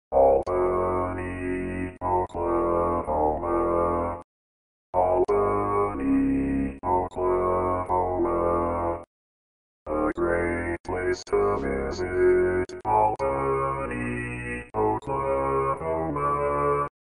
How to Say or Pronounce USA Cities — Albany, Oklahoma
This video shows you how to say or pronounce Albany, Oklahoma.

A computer said Albany, Oklahoma. How would you say Albany, Oklahoma?